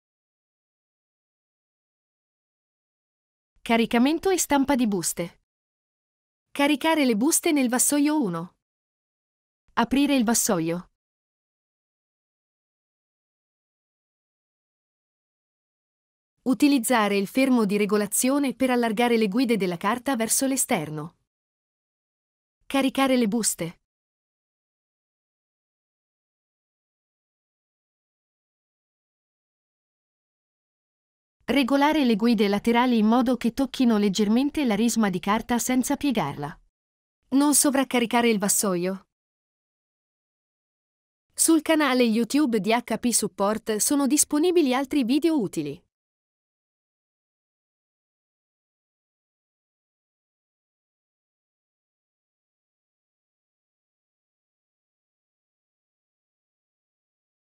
Caricamento e stampa di buste | HP CLJ 57/6700, X557/654 e multifunzione 58/6800, X579/677
Informazioni su come caricare e stampare le buste nelle stampanti HP Color LaserJet 5700, X557, 6700, 6701, X654 e multifunzione 5800, X57945, X58045, 6800, 6801, X677.

Caricamento e stampa di buste | Stampanti HP Color LaserJet 57/6700, X557/654 e multifunzione 58/6800, X579/677 | HP Support